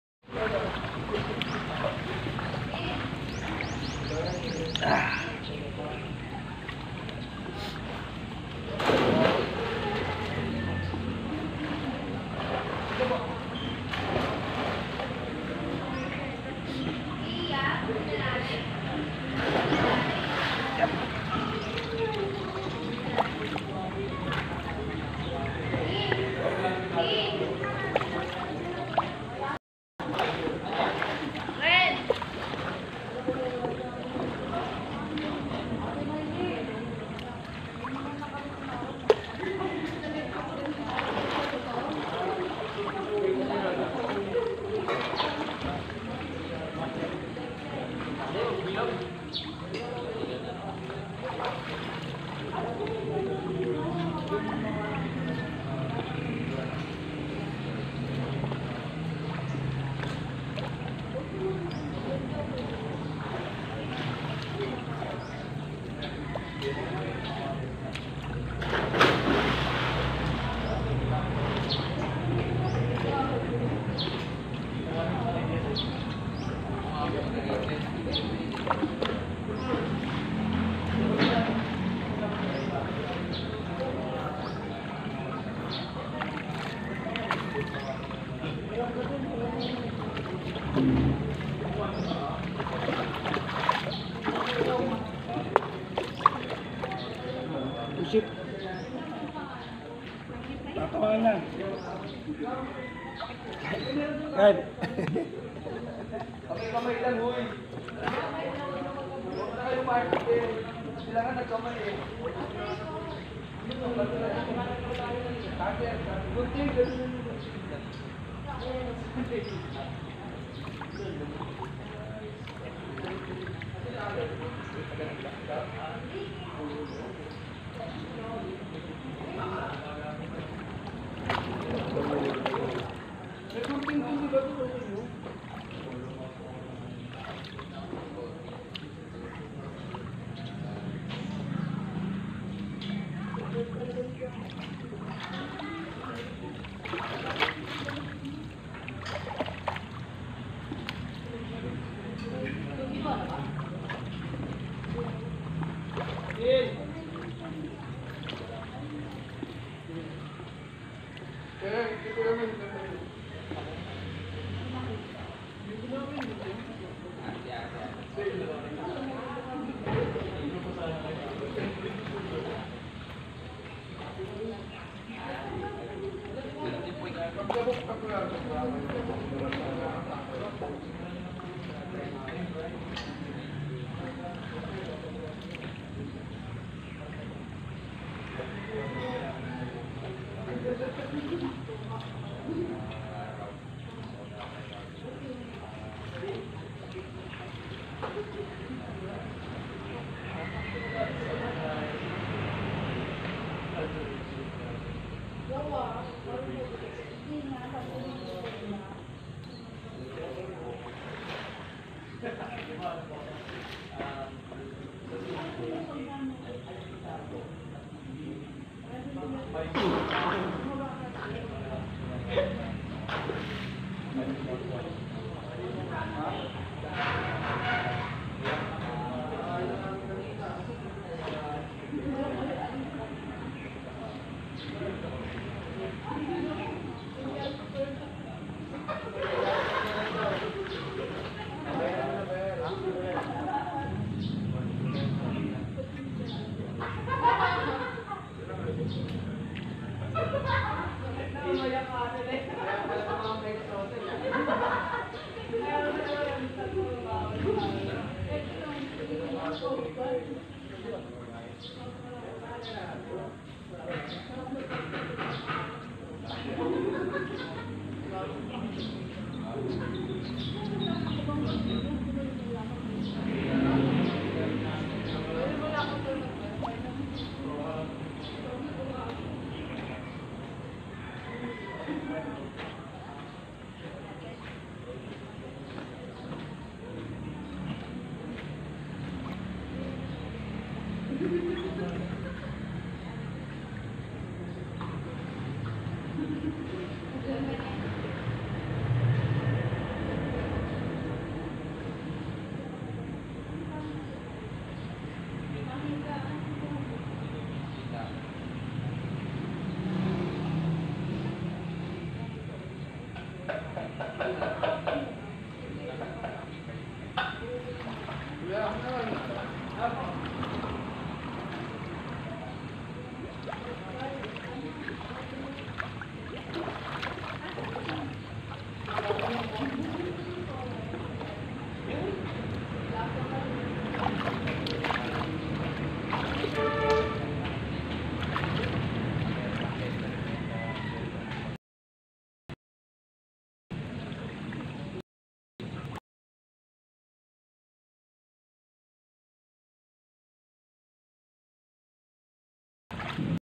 Swimming on my 50th Birthday (Late upload)
I seldom have this simple swimming pleasure for myself that has been busy all the time. My wife Maricon surprised me on my 50th birthday at Tito Renz Norzagaray... A simple celebration yet so memorable to have for a lifetime. Thanks Hon Maricon and Kyle.